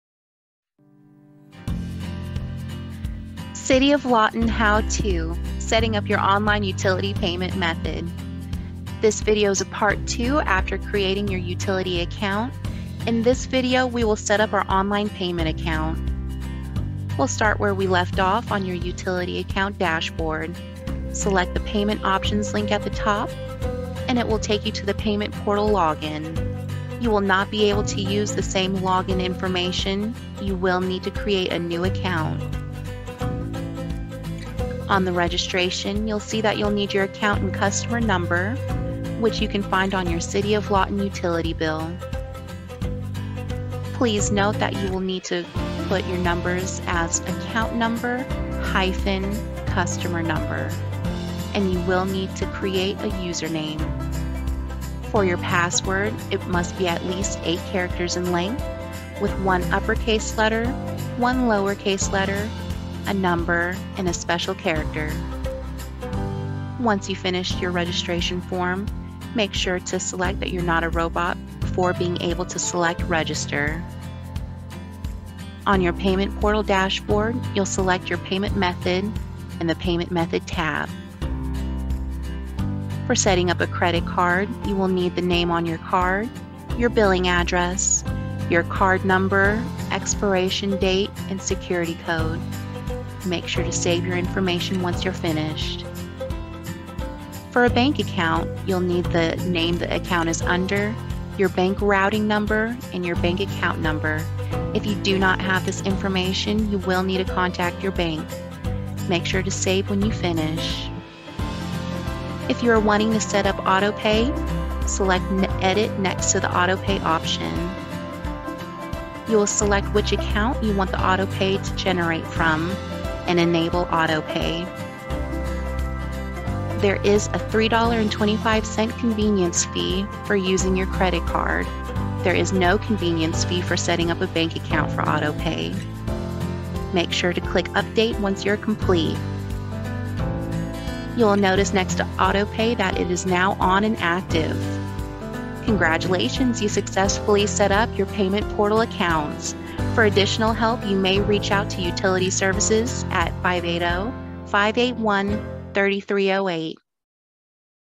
COL Utility Billing How To Part 2
This how to video explains the process for signing up within with Payment Portal of the City of Lawton's new utility billing software. Set up a Payment Portal account after creating a Self Service Portal account by going

If you haven't created a Self Service Portal account, be sure to check out part one of this video series

More information about utility billing software conversion is available on the city's

If citizens need further assistance on setting up accounts online, they may call , which is a designated line to assist citizens with accessing their account number and customer number or to seek technical assistance with the portal. Assistance at this number will be available from 8 a.m. to 5 p.m. Monday-Friday. Due to high call volume, citizens may experience prolonged wait times.